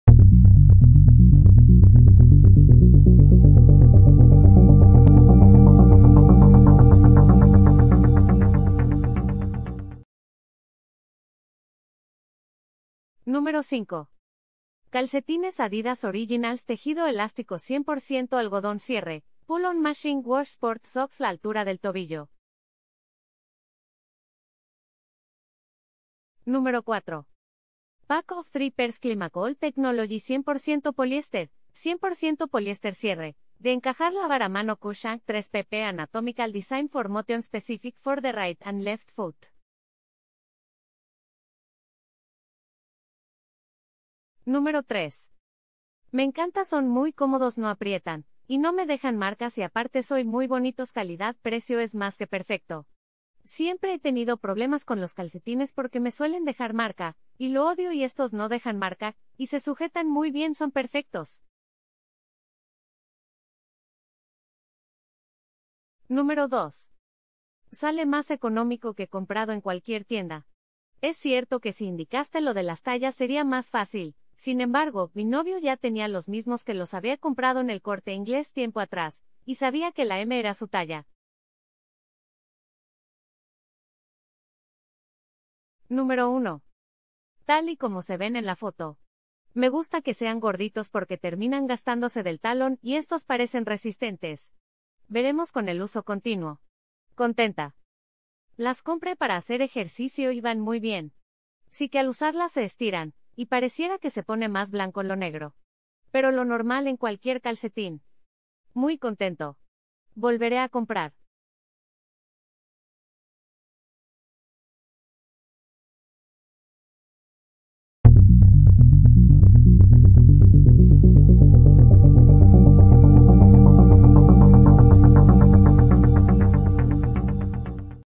Los 5 mejores calcetines adidas blancos
✋  Aquí están los 5 mejores calcetines adidas blancos disponibles en el mercado.

🔽🔽🔽 ENLACES DE COMPRA CALCETINES ADIDAS BLANCOS 🔽🔽🔽

5) adidas Fold Cuff Crw Socks Unisex adulto

4) adidas Cush Ank 3pp Calcetines Hombre

3) adidas Light Crew 3pp Calcetines, Unisex Adulto

2) adidas Cush CRW 6pp Calcetines, Unisex Adulto

1) adidas 3s CSH Crw3p Calcetines Unisex Adulto

00:00 - Intro
00:13 - 5) adidas Fold Cuff Crw Socks Unisex adulto...
00:26 - 4) adidas Cush Ank 3pp Calcetines Hombre...
00:44 - 3) adidas Light Crew 3pp Calcetines, Unisex Adulto...
01:05 - 2) adidas Cush CRW 6pp Calcetines, Unisex Adulto...
01:25 - 1) adidas 3s CSH Crw3p Calcetines Unisex Adulto...